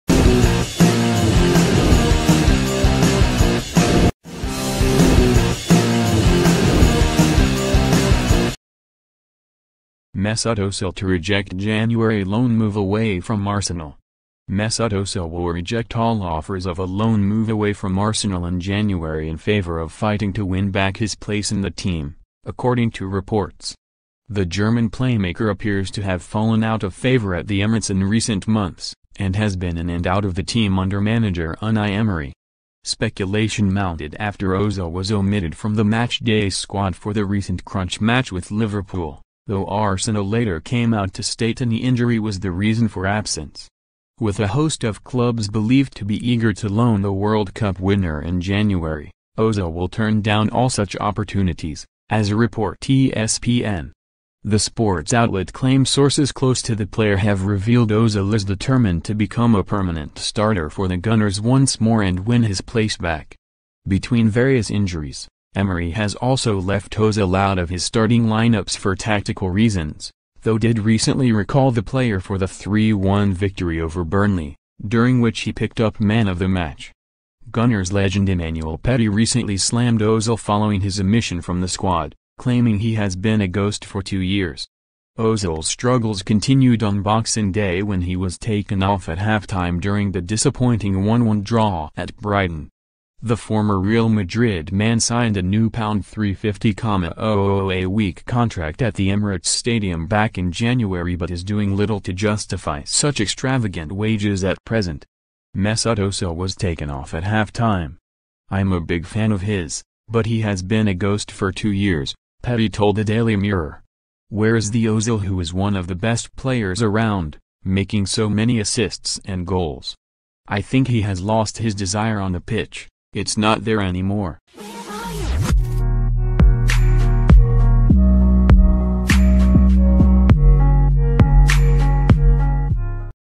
Mesut Ozil to 'reject January loan move away from Arsenal'
Mesut Ozil will reject all offers of a loan move away from Arsenal in January in favour of fighting to win back his place in the team, according to reports.

The German playmaker appears to have fallen out of favour at the Emirates in recent months, and has been in and out of the team under manager Unai Emery.

Speculation mounted after Ozil was omitted from the match-day squad for the recent crunch match with Liverpool, though Arsenal later came out to state a knee injury was the reason for absence.

With a host of clubs believed to be eager to loan the World Cup winner in January, Ozil will turn down all such opportunities, as report ESPN.

The sports outlet claim sources close to the player have revealed Ozil is determined to become a permanent starter for the Gunners once more and win his place back.

Between various injuries, Emery has also left Ozil out of his starting line-ups for 'tactical reasons,' though did recently recall the player for the 3-1 victory over Burnley - during which he picked up Man of the Match.

Gunners legend Emmanuel Petit recently slammed Ozil following his omission from the squad, claiming 'he has been a ghost for two years.'

Ozil's struggles continued on Boxing Day when he was taken off at half-time during the disappointing 1-1 draw at Brighton.

The former Real Madrid man signed a new £350,000-a-week contract at the Emirates Stadium back in January but is doing little to justify such extravagant wages at present.

'Mesut Ozil was taken off at half-time. I'm a big fan of his, but he has been a ghost for two years,' Petit told the Daily Mirror.

'Where is the Ozil who was one of the best players around, making so many assists and goals? I think he has lost his desire on the pitch, it's not there any more.'